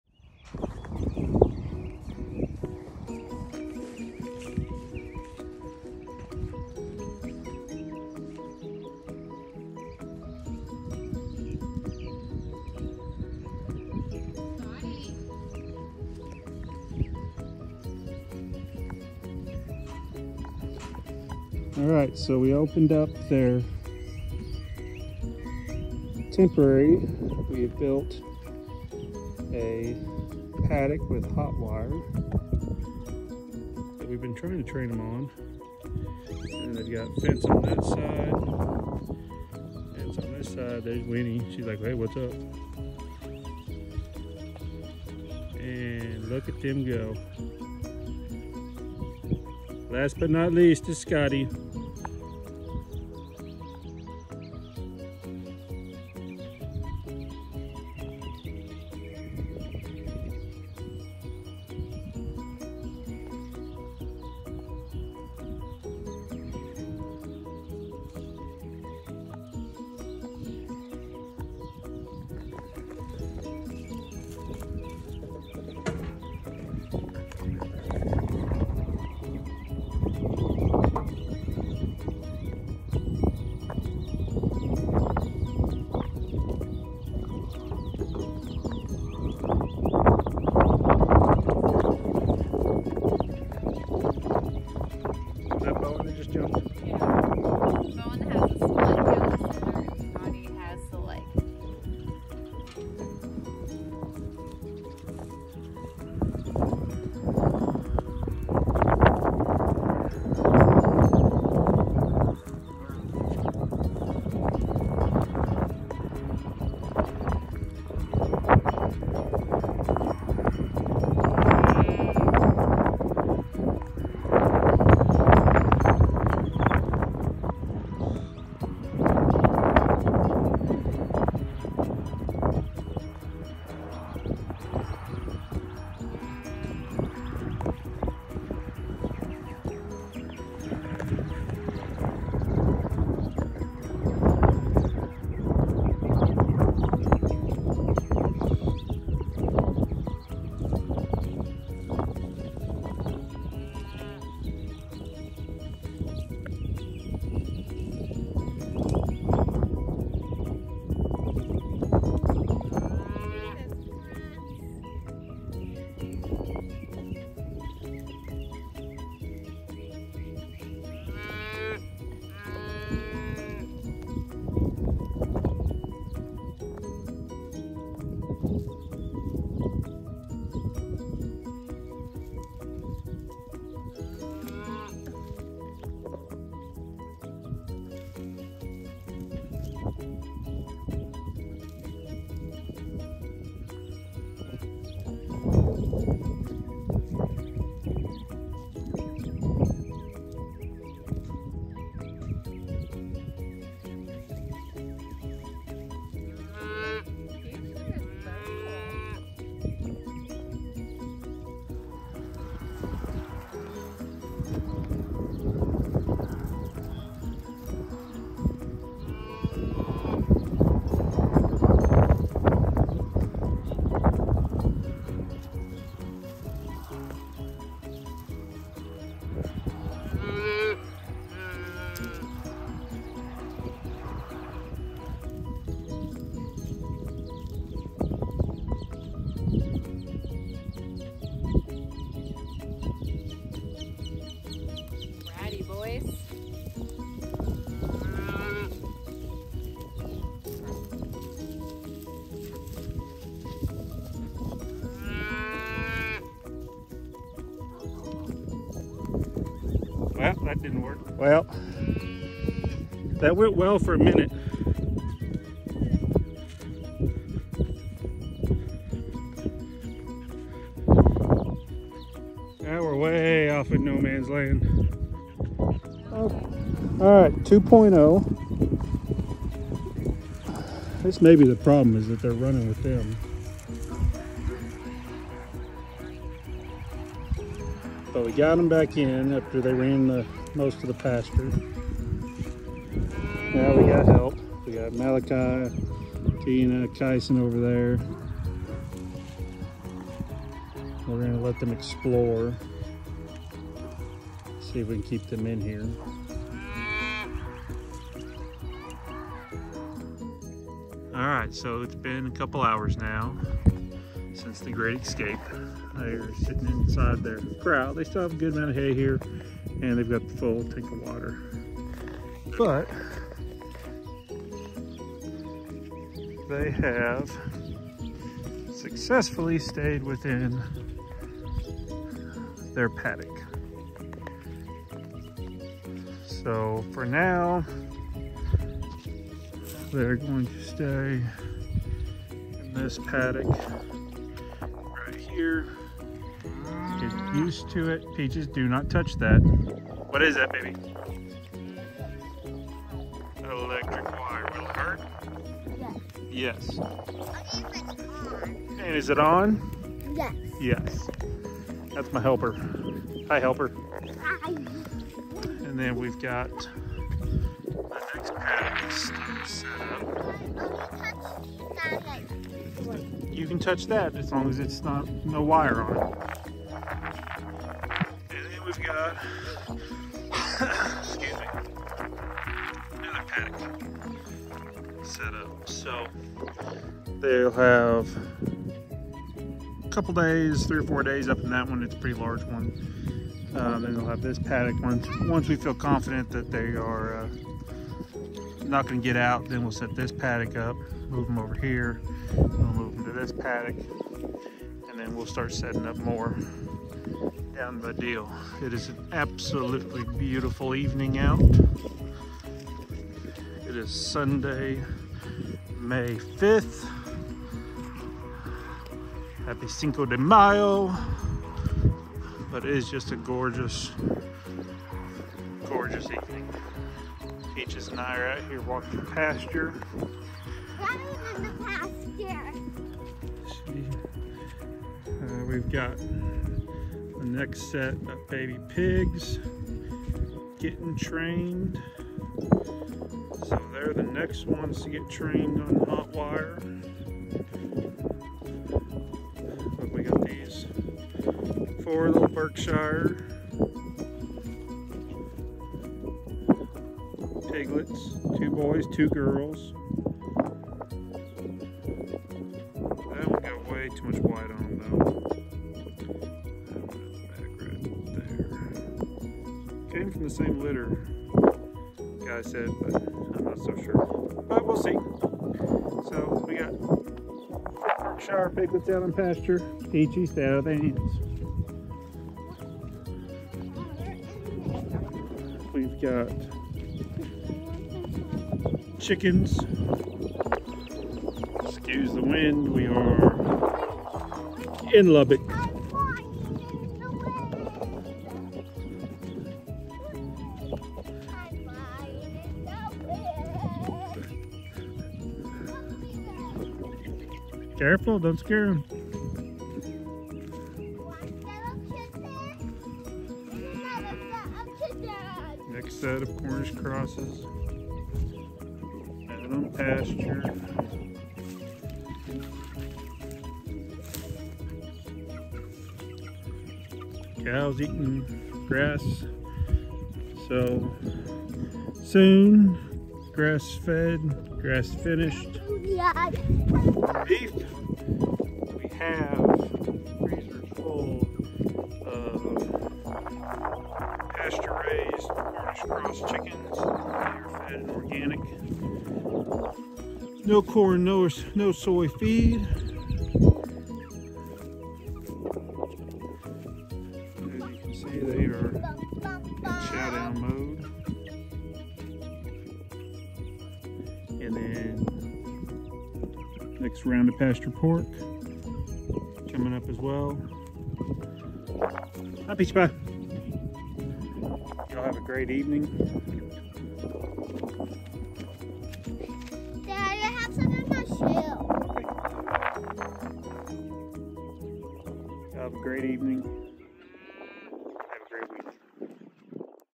Moving our first Dexter’s on to the pasture!
After a couple weeks getting them used to our farm we are starting the Dexter steers out on their pasture rotations!